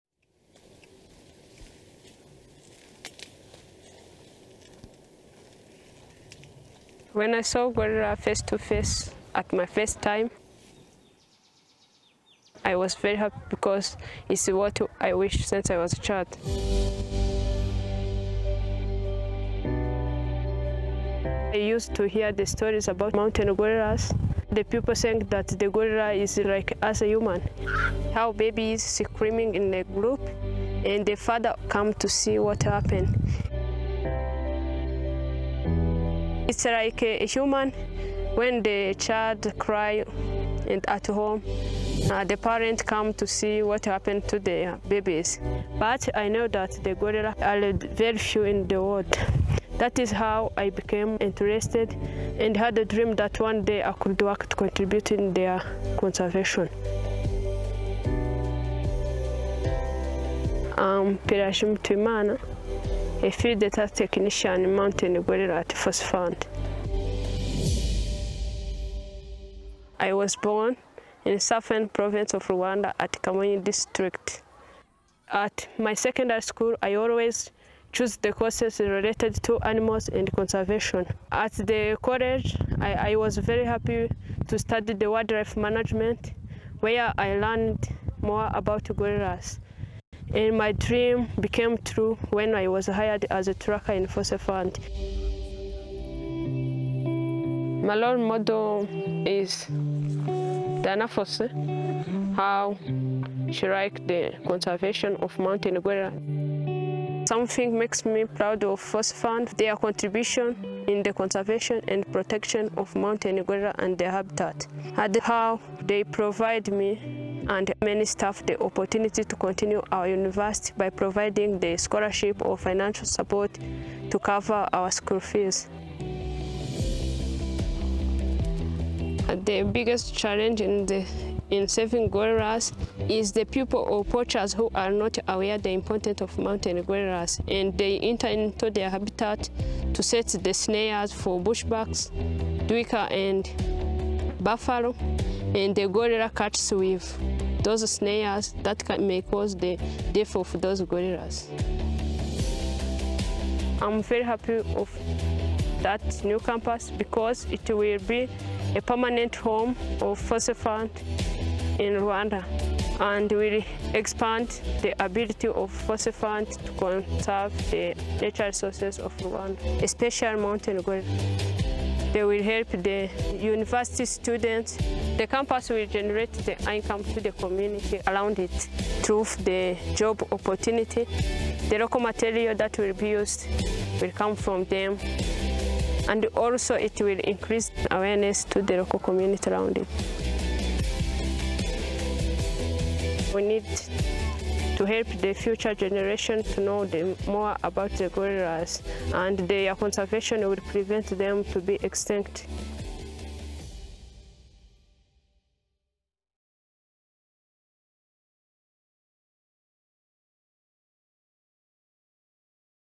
Gorilla Game Changer Pelagie Mutuyimana
Meet Gorilla Game Changer Pelagie Mutuyimana, Gorilla Tracker at Karisoke Research Center.

Pelagie started with the Fossey Fund as an intern in 2017 and was hired one year later as a tracker. Pelagie is one of only two female trackers at Karisoke!  In addition to her work in the field, Pelagie is also studying to get her bachelor’s degree through support from the Fossey Fund. Her favorite gorillas are subadult male Ndizeye and silverback Segasira, both from Titus’s group.